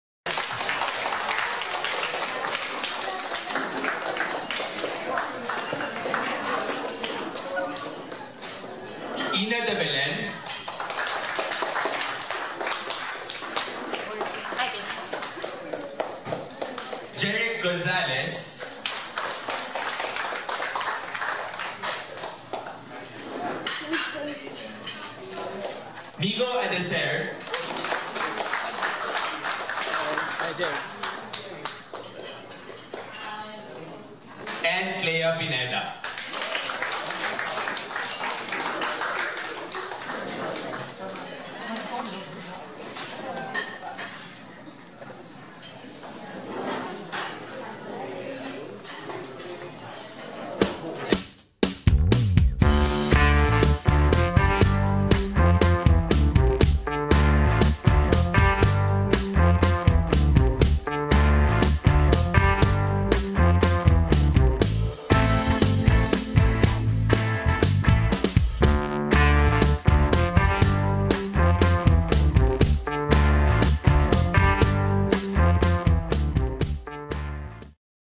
Meet the Junior Cast of GMA Drama's IKA-5 UTOS
The cast of Ika-5 Utos - Migo Adecer, Klea Pineda, Jeric Gonzales, Inah De Belen and Jhake Vargas